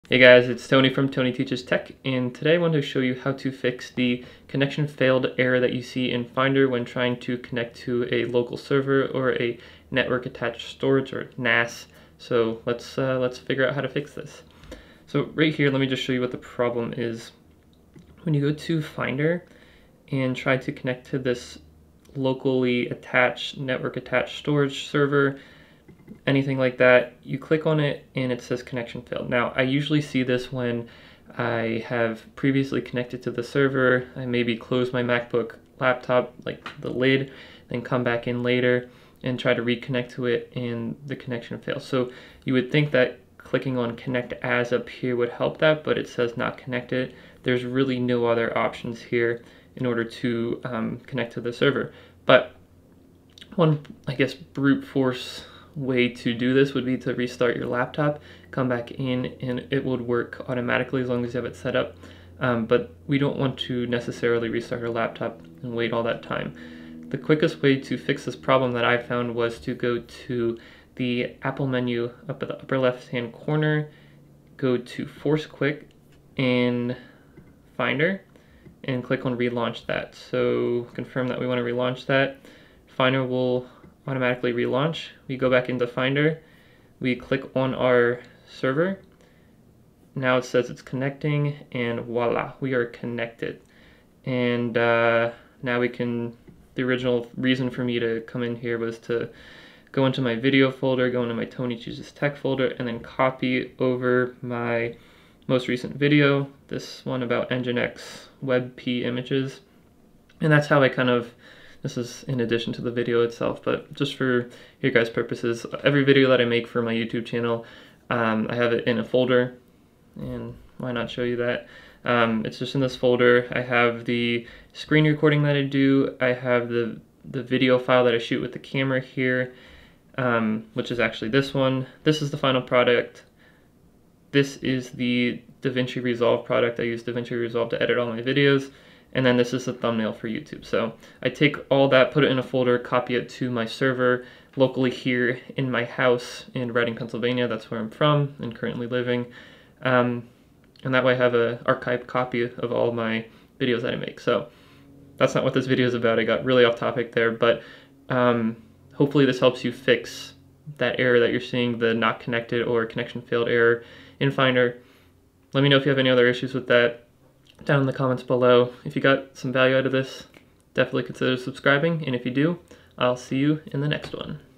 How to Fix Server Connection Failed Error in Mac Finder Without Reboot
Learn how to fix the Mac "Connection Failed" and "Not Connected" error in Finder when trying to connect to a server or NAS. This simple
 fix does not involve rebooting your Mac, but instead simply relaunching Finder.